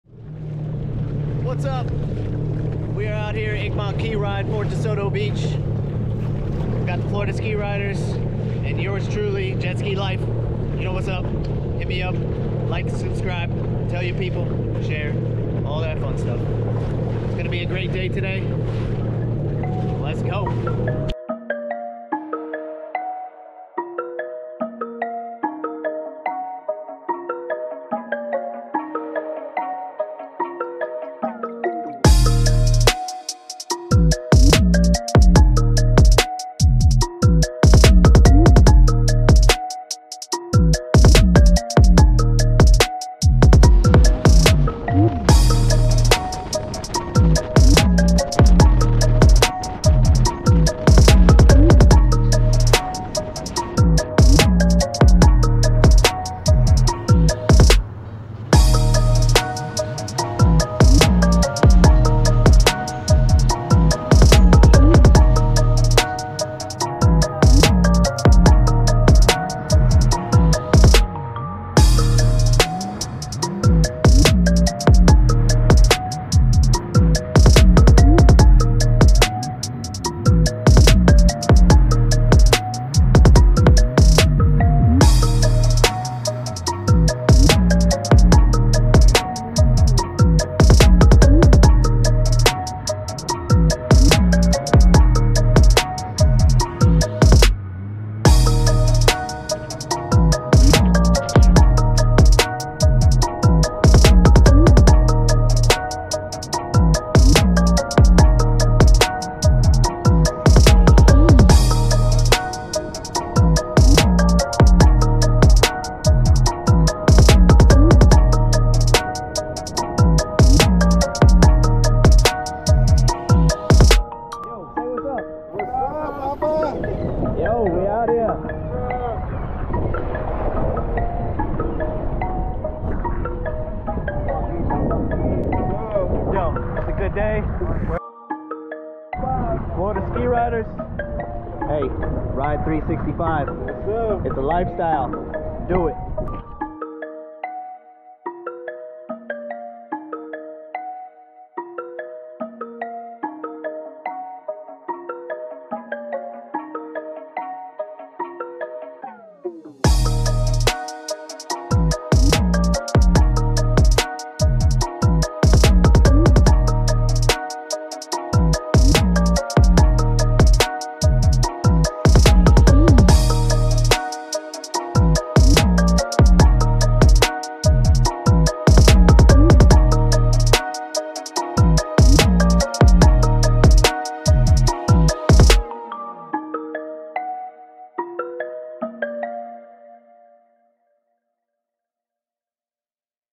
What It's Like To Ride With 100 Jet Ski Riders
Florida Ski Riders Egmont Key Invasion. Watch as we ride to Passage Key and Egmont Key with 100 other Ski Riders.